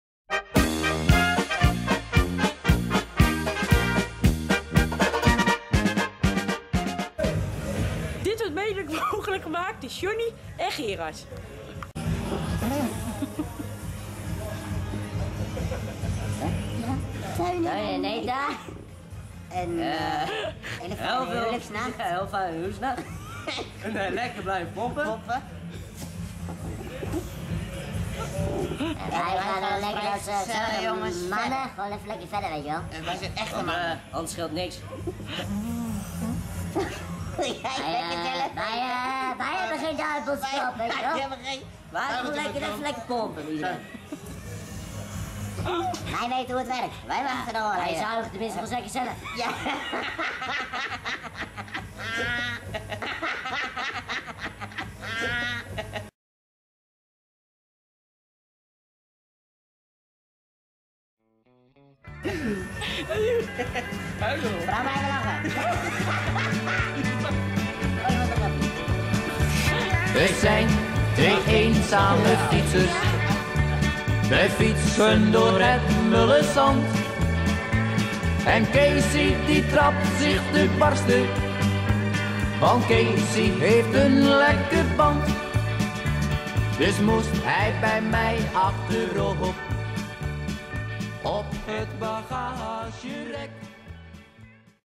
de eenzame vrijgezellen
de eenzame vrijgezellen doen een stukje voor de camera  met een beetje helium in het huigje.